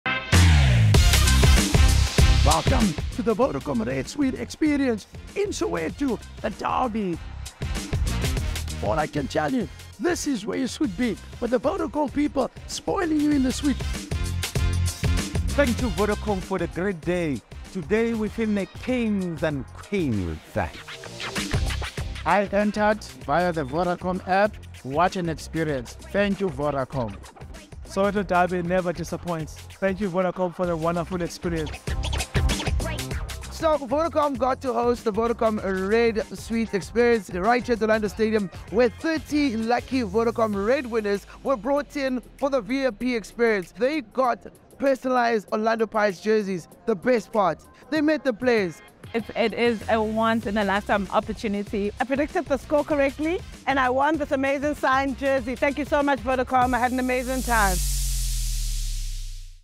Our RED clients scored an unforgettable sweet suite experience.
We treated our Vodacom RED clients with back-to-back suite experiences at FNB and Orlando Stadium, starting with the legendary Soweto Derby and ending off with Orlando Pirates facing off against league neighbours, SuperSport United.